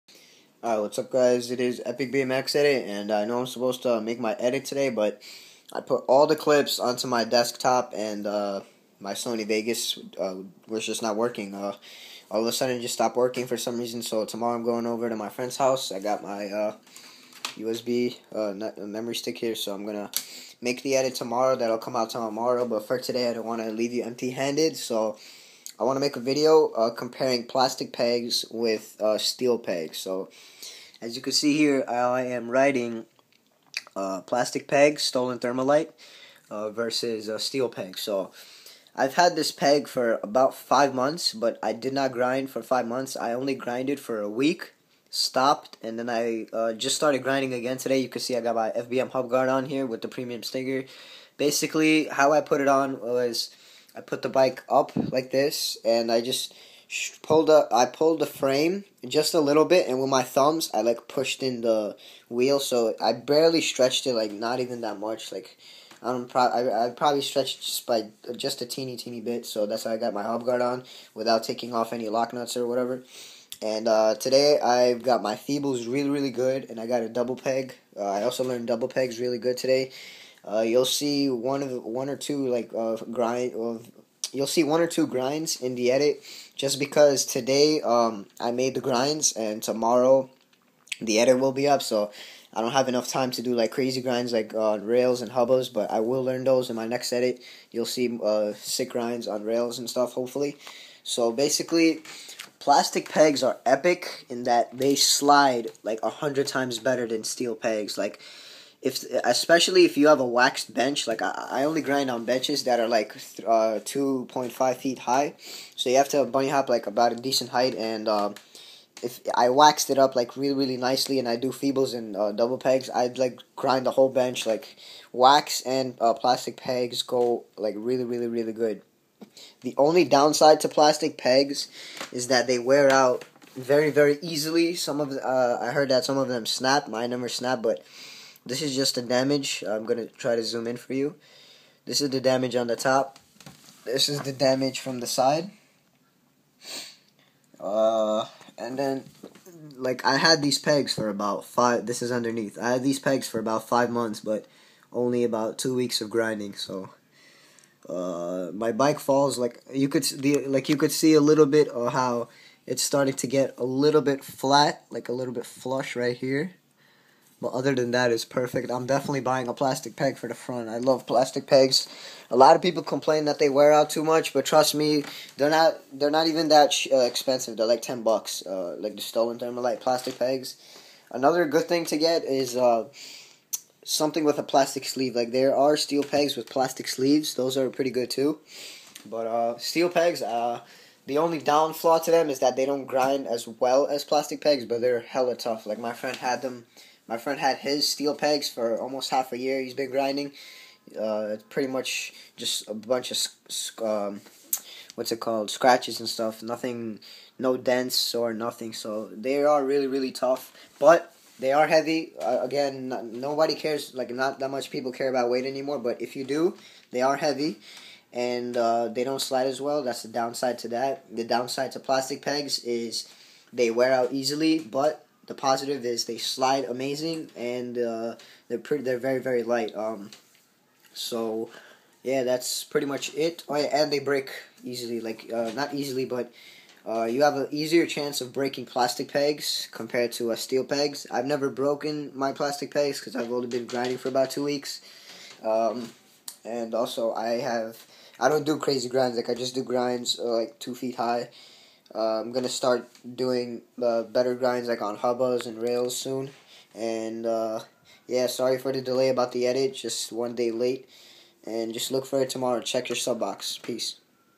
Bmx plastic pegs vs Steel pegs
i am comparing plastic pegs vs steel say what u guys want but everyone has there personal prefrence. i am just stating facts steel pegs take super long to wear down plastic pegs wear down quicker but slide better